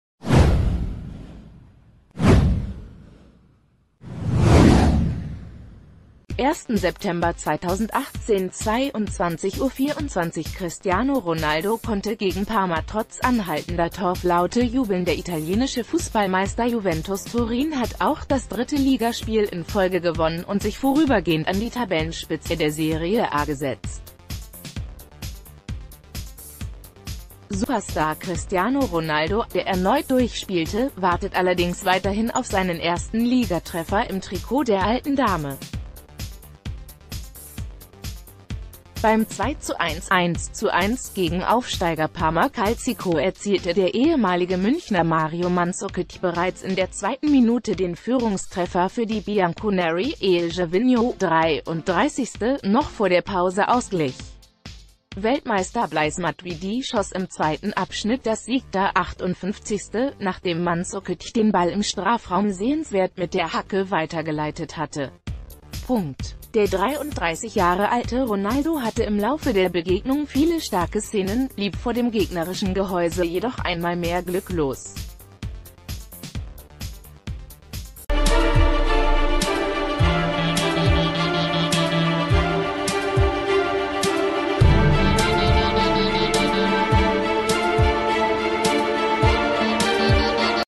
Juventus feiert nächsten Sieg - CR7-Durststrecke hält an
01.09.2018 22:24 Cristiano Ronaldo konnte gegen Parma trotz anhaltender Torflaute jubeln Der italienische Fußball-Meister Juventus Turin hat auch das dritte Ligaspiel in Folge gewonnen und sich vorübergehend an die Tabellenspitze der Serie A gesetzt. Superstar Cristiano Ronaldo, der erneut durchspielte, wartet allerdings weiterhin auf seinen ersten Ligatreffer im Trikot der Alten Dame. Beim 2:1 (1:1) gegen Aufsteiger Parma Calcio erzielte der ehemalige Münchner Mario Mandzukic bereits in der zwe...